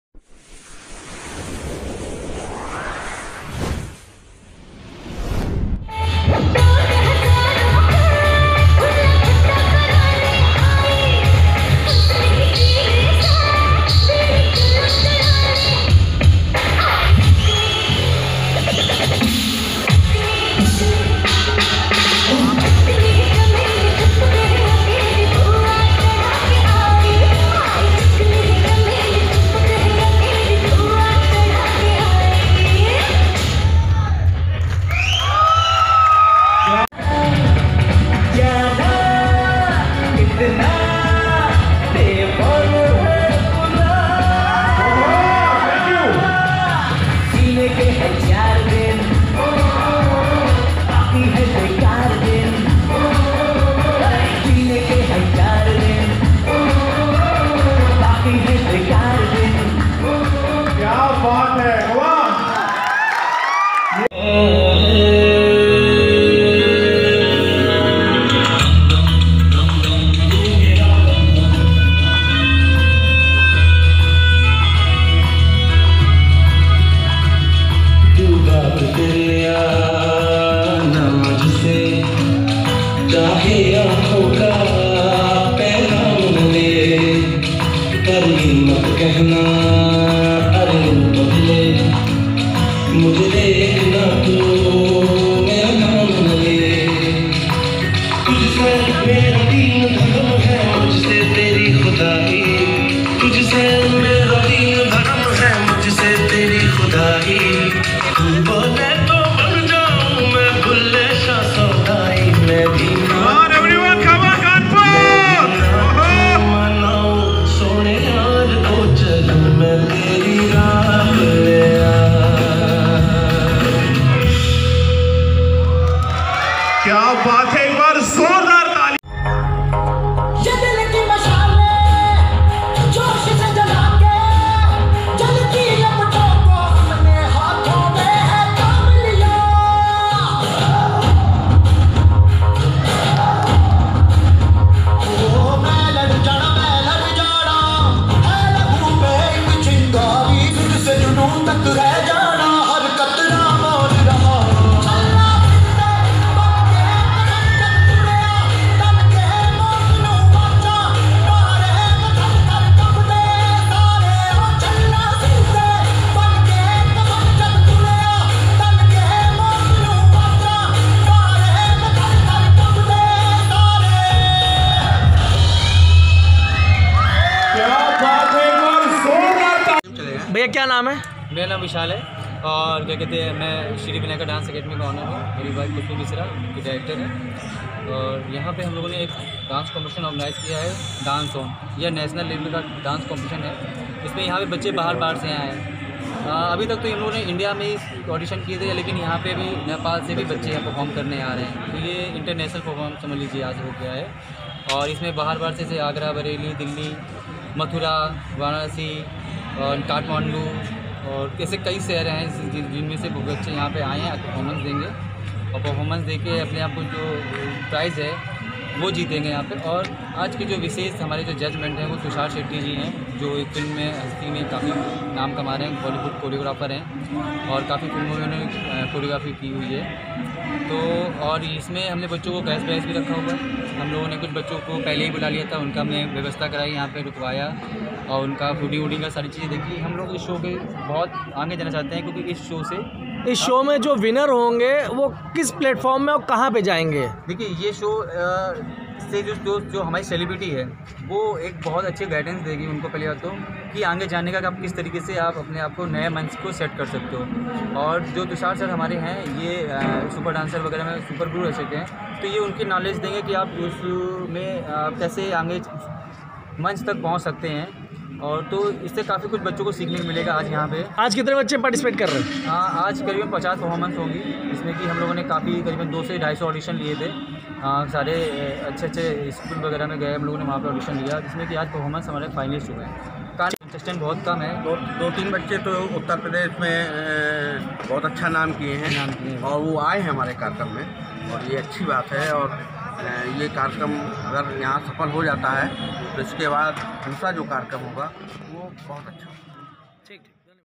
*हुनर ही विनर है* *जानकारी जंक्शन कानपुर पत्रकार अब्दुल निसार* श्री विनायक कानपुर डांस सेंटर द्वारा
*हुनर ही विनर है*
*जानकारी जंक्शन कानपुर पत्रकार अब्दुल निसार*
श्री विनायक कानपुर डांस सेंटर द्वारा लाजपत भवन में डांस ऑन का फाइनल हुआ जिसमें बच्चों ने एक से बढ़कर एक प्रतियोगिता दी भाग लेने के लिए कई शहरों से बच्चे आए जैसे मेरठ आगरा बरेली प्रयागराज इटावा आदि ।डांस प्रतियोगिता का आयोजन श्री विनायक शिक्षा संस्थान द्वारा किया गया संस्था की डायरेक्टर खुशबू मिश्रा ने बताया कि बच्चों के 1 माह से ऑडिशन चल रहे थे जिसका आज पहला ऑडिशन एबीएस स्टूडियो में संपन्न हुआ दूसरा ऑडिशन हवेली गार्डन एवं सेमीफाइनल 5 जनवरी फिटनेस मंत्रा में आयोजित हुआ फाइनल में विजेता पहला स्थान जूनियर में रिहान वर्मा द्वितीय अनुष्का का और अनामिका तृतीय स्थान वनी और अंकित ने प्राप्त किया सीनियर ने प्रथम स्थान कार्तिक पांडे द्वितीय स्थान रवि शर्मा ऋषभ रावल विजेताओं को डांस ऑन ट्रॉफी व नगद पुरस्कार देकर सम्मानित किया गया। कार्यक्रम में मुख्य अतिथि माननीय प्रमिला पांडे जी महापौर कानपुर नगर ने सभी प्रतियोगी बच्चों को आशीर्वाद दिया आगे बढ़ने की शुभकामनाएं दी इस अवसर पर डायरेक्टर खुशबू मिश्रा विशाल सर नीलेश कौशिक अंकित एंकर प्रतीक विकास हामिद नदीम सुशील सोनी आदि लोग उपस्थित रहे।